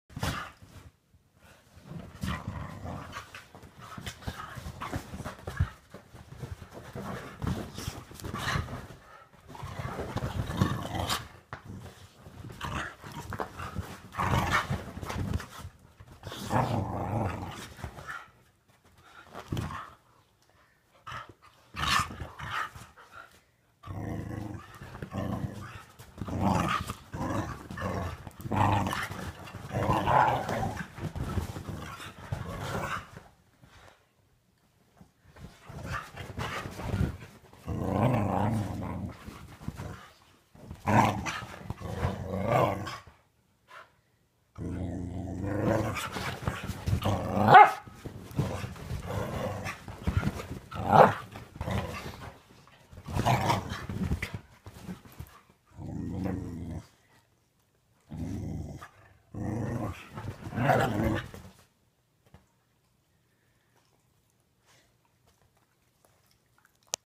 Mega Scrap 2000
See, Tig plays too. Having a fun play session on the couch. "We can't have nice things". Shoddy quality, but had to really lighten this up so anything could be seen in the darkness.